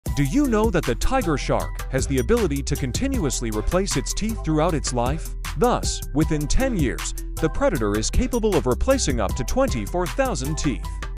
Tooth Replacement Ability of the Tiger Shark
Do you know that the Tiger Shark has the ability to continuously replace its teeth throughout its life?
Photos:
Scarface-tigershark.jpg From Wikimedia Commons
Juvenile tiger shark Bahama's by Albert kok.
Tiger shark teeth.jpg by Stefan Kühn